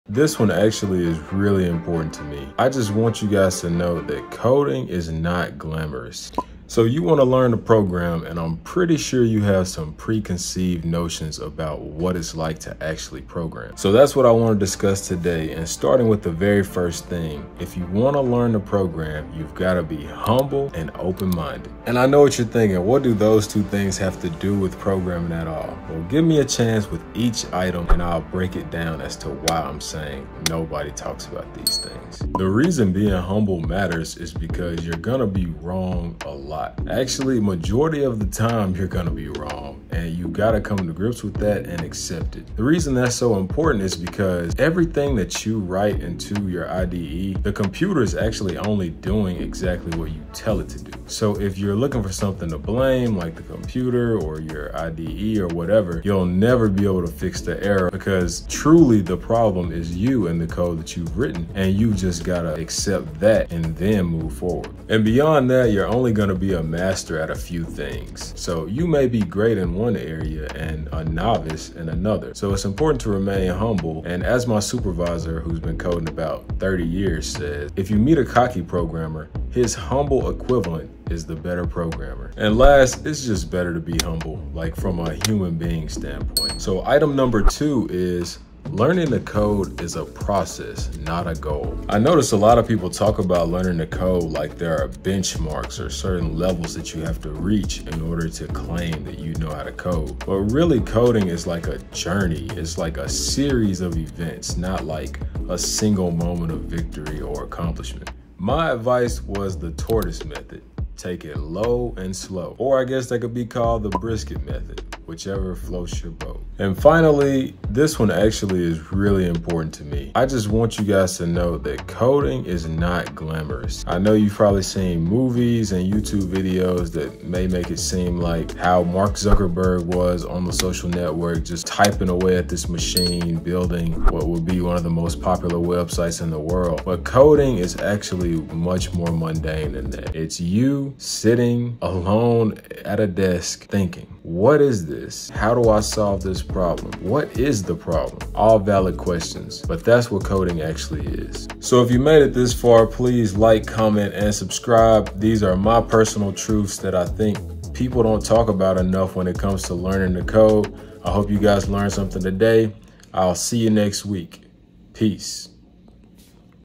3 TRUTHS No One Tells You About Learning To Code
Youtubers have lied to you. Learning to code is not all fun and games. The end result can help you build a wonderful life for yourself and your family, but in this video, I pull back the curtain on many misconceptions about coding that have been promoted the past few years. My hope is that this insight I offer can help you along in your journey and adjust your expectations. - So So Dev

MY TECH:

MY KEYBOARD (FEEL + SOUND IS EXCELLENT):

MY CAMERA FOR YOUTUBE:

BEST PRODUCTIVITY MOUSE: